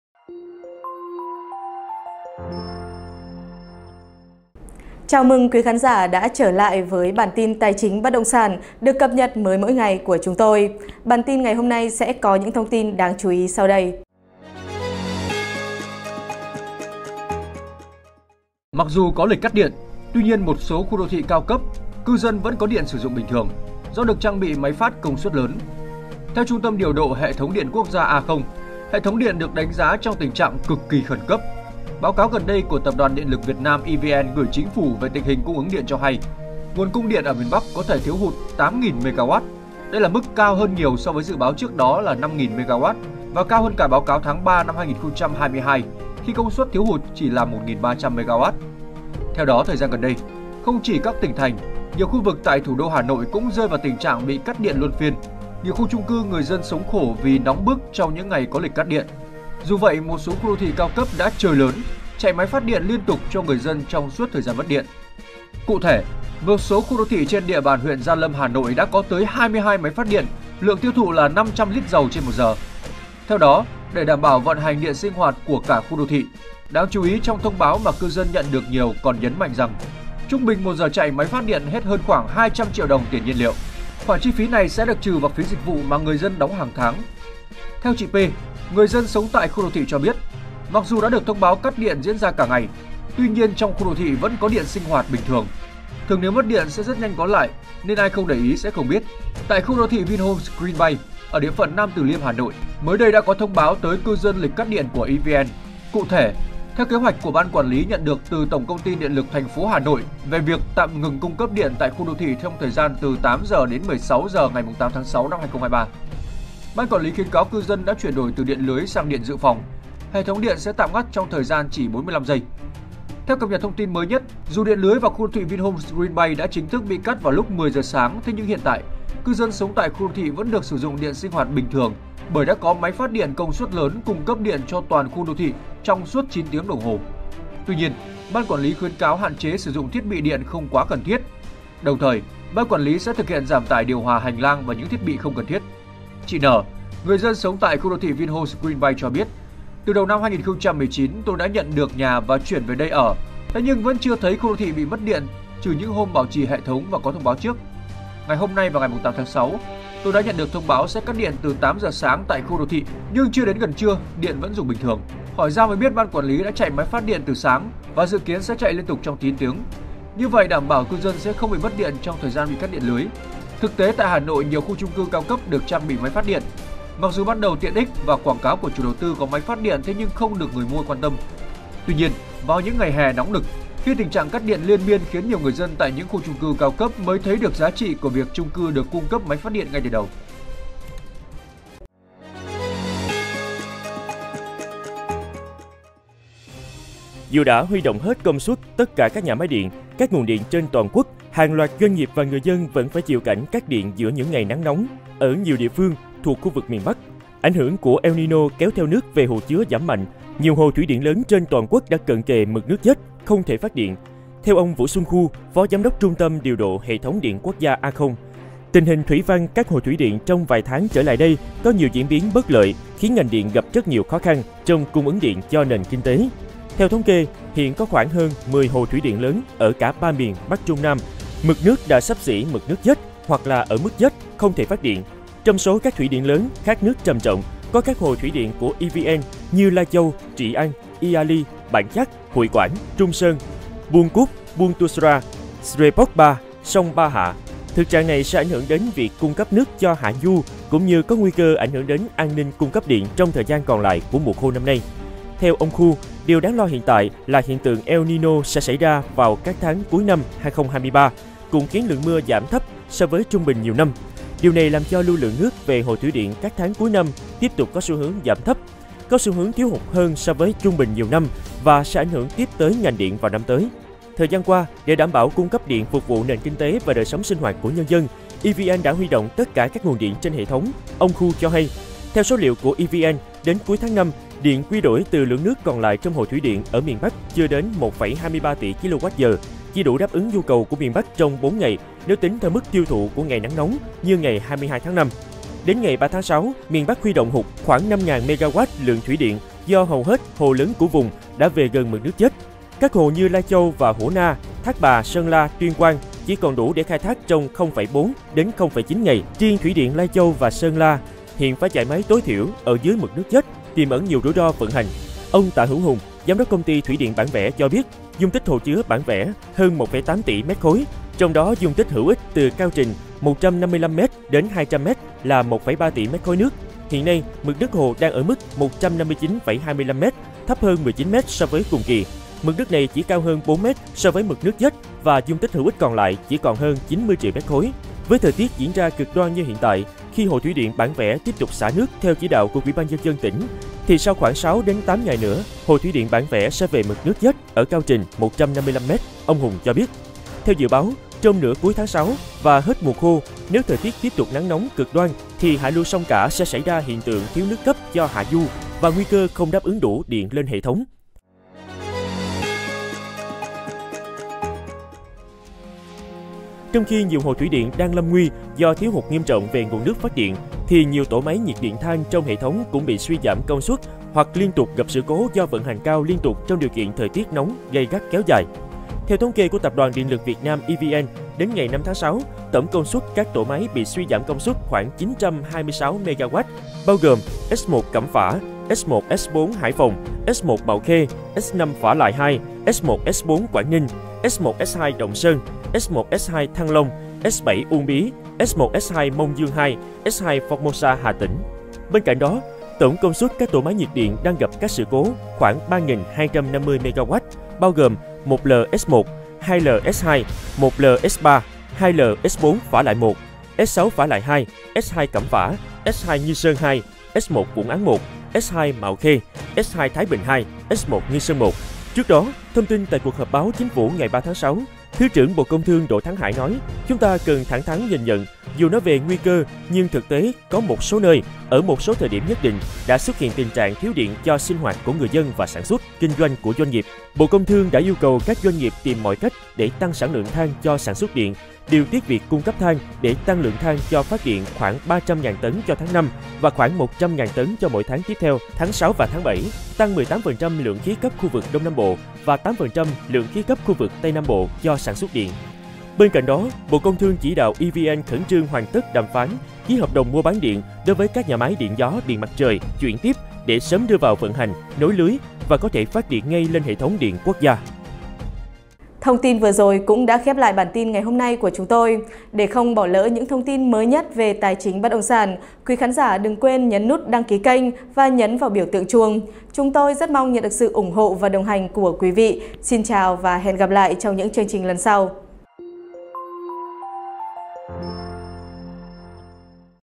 Cư dân ở khu cao cấp: nắng nóng đỉnh điểm không lo mất điện, chạy máy phát 200 triệu/h | CafeLand
Cư dân ở khu cao cấp: nắng nóng đỉnh điểm không lo mất điện, chạy máy phát 200 triệu/h

Mặc dù có lịch cắt điện, tuy nhiên một số khu đô thị cao cấp cư dân vẫn có điện sử dụng bình thường, do được trang bị máy phát công suất lớn.